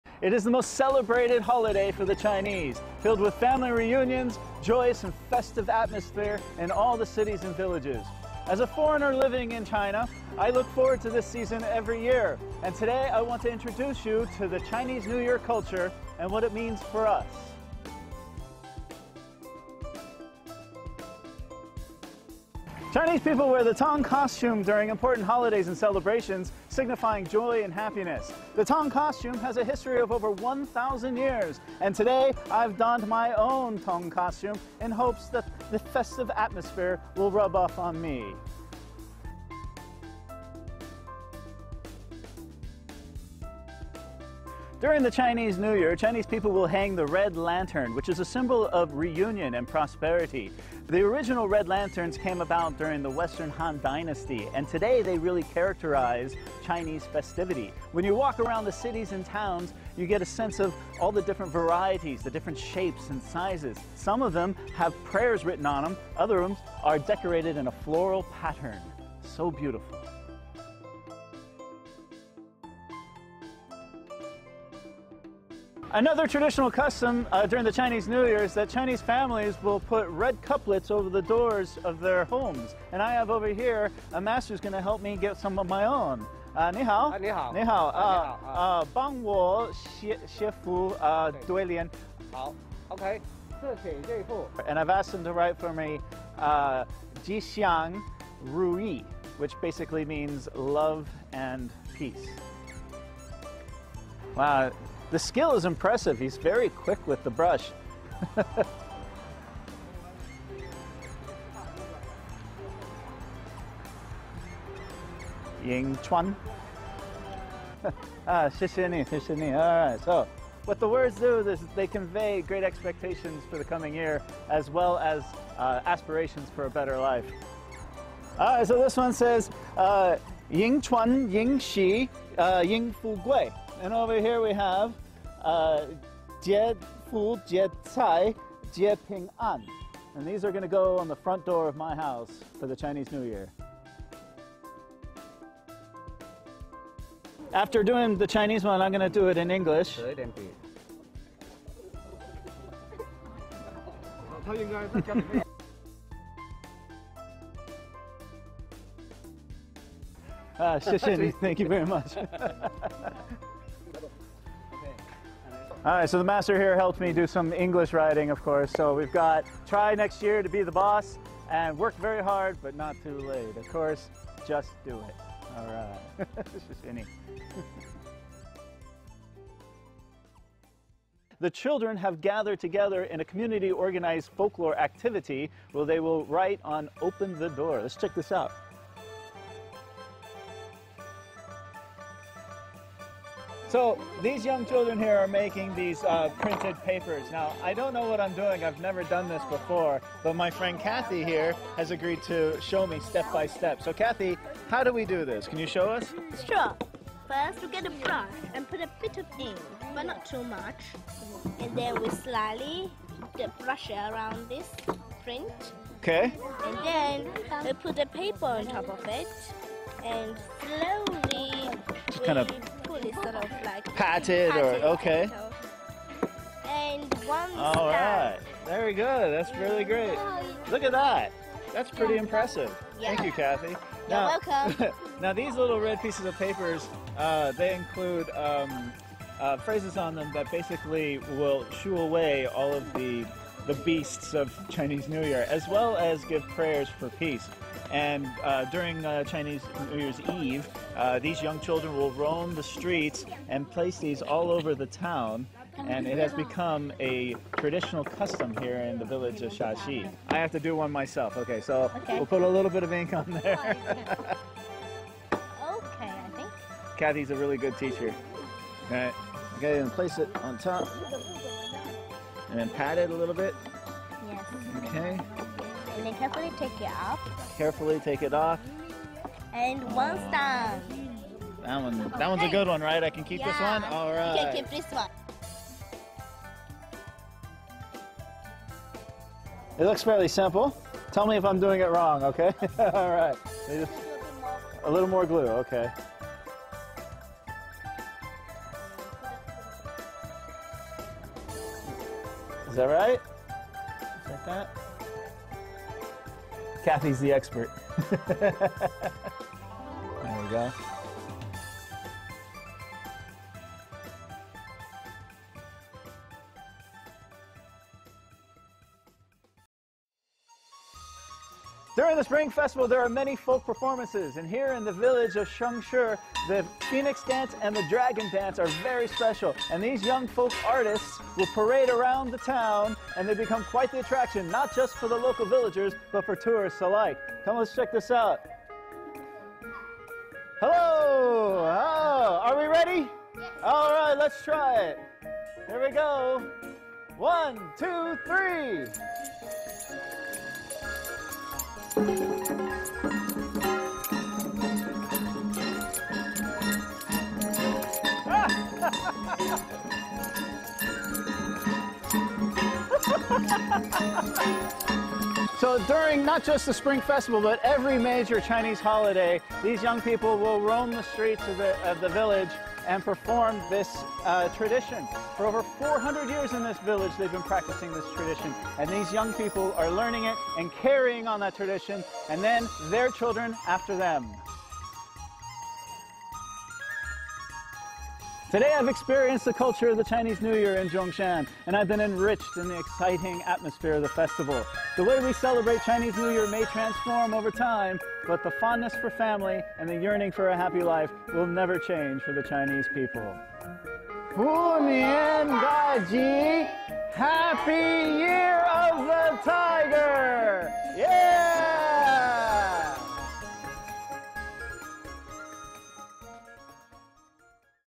GLOBALink | Vlog: American expat explores Chinese New Year's traditions in S China
Wearing Tang suit, writing couplets, making printed papers... An American expat living in China's Zhongshan has explored some traditional culture and customs to celebrate Chinese New Year.